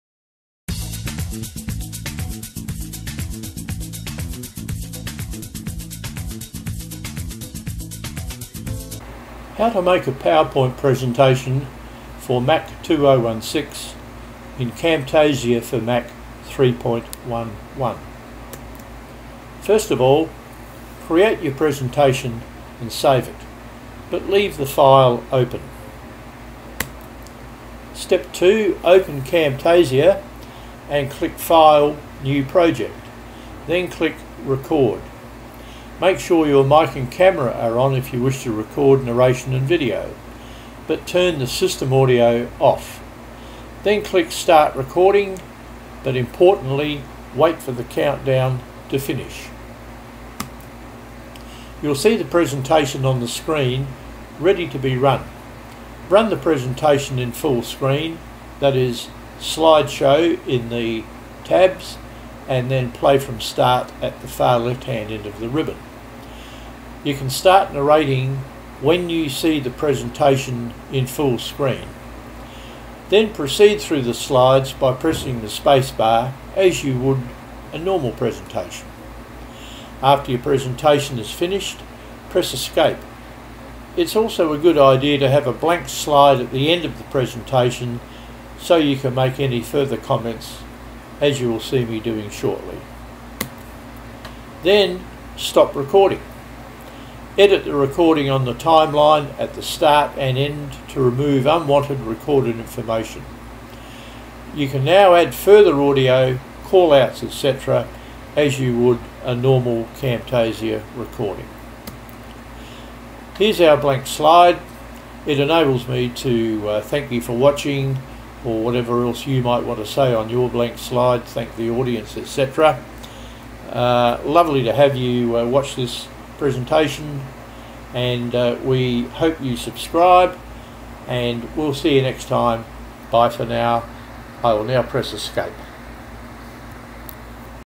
Create a Powerpoint presentation in Camtasia for Mac. Also for Windows versions
Camtasia have not made available a Powerpoint add-in their Mac version.
This video will explain how to overcome this shortcoming in a few easy steps.
Great for corporate, training and all types of PowerPoint presentations using PowerPoint 2016 for Mac